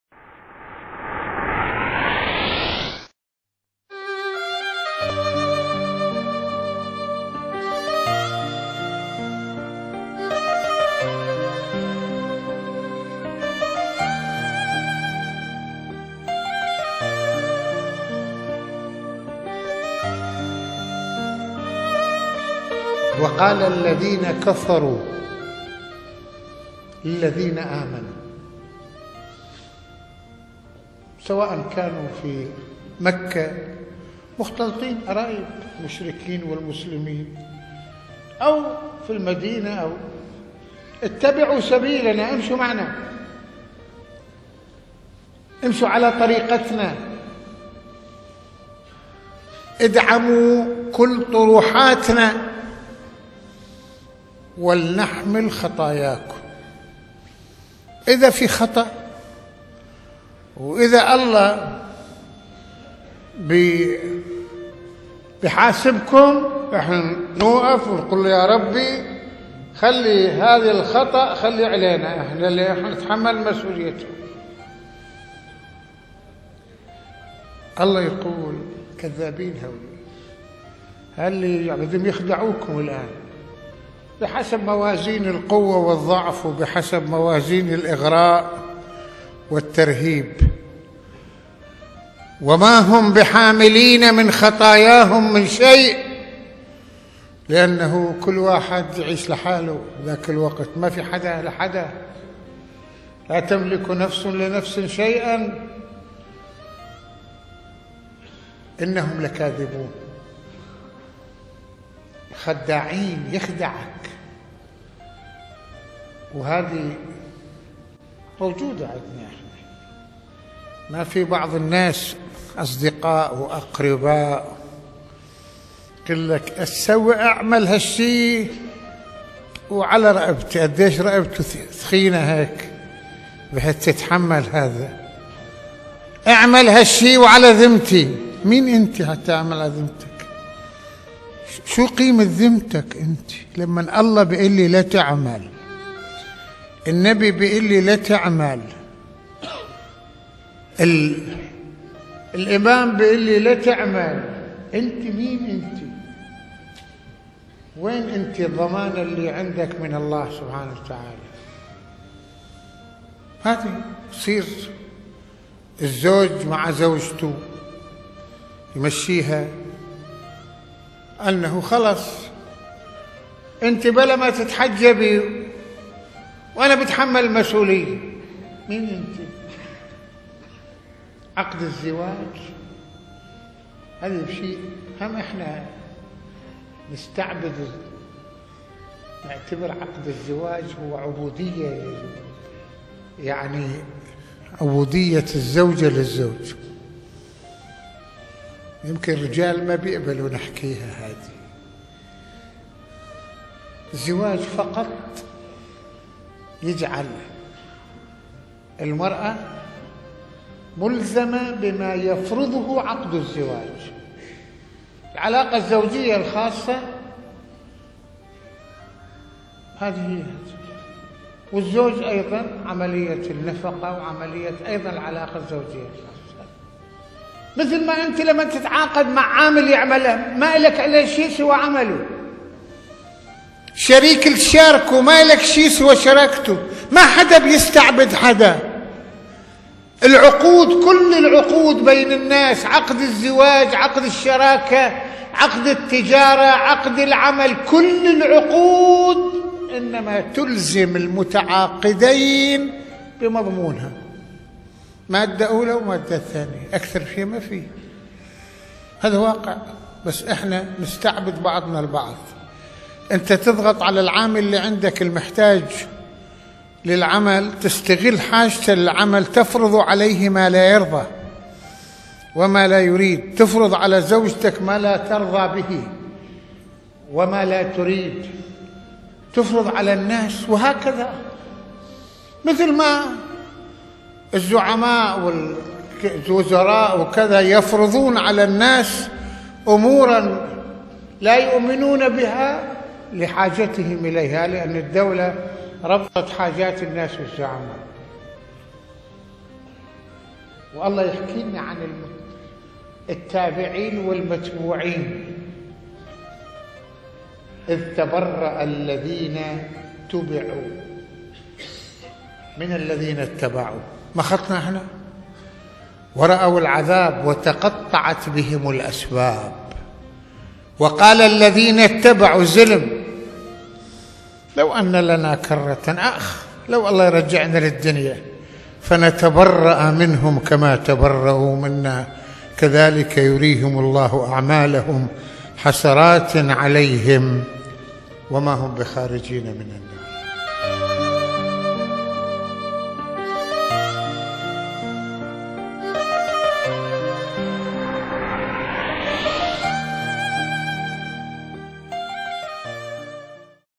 لا أحد يحمل خطايا أحد / No one shall bear the sins of others
Francais:
Dans cette vidéo, Son Eminence, l'Autorité religieuse Sayed Mohammad Hussein Fadlallah (ra), interprète un verset coranique par lequel il explique que les mécréants qui appellent les croyants à entreprendre le chemin de l'égarement avec eux, affirmant qu'ils assumeront les conséquences de leurs péchés ne sont que des menteurs et des imposteurs. Il conclut que personne ne peut supporter les conséquences des péchés et des fautes des autres.

English:
In this video, His Eminence, the late Religious Authority, Sayyed Muhammad Hussein Fadlullah (ra), interprets a Quranic Ayah by which he explains that the disbelievers who call on the believers to undertake the road to misguidance with them, claiming that they will bear the consequences of their sins are but liars and deceivers. He concludes that no one can bear the consequences of the sins and faults of the others.

عربي:
يشرح سماحة المرجع السيّد محمد حسين فضل الله(رض) في هذا المقطع آية قرآنيّة، مبيِّناً من خلالها ما يقوم به الكفّار الّذين يدعون المؤمنين إلى سلوك طريق الضّلال معهم، مدّعين بأنّهم سيتحمّلون كلّ الخطايا عنهم، ولكنّ سماحته يشير إلى أنّ الله سبحانه يؤكّد أنَّ هؤلاء كاذبون وخادعون، لأن لا أحد يتحمَّل خطايا أحد...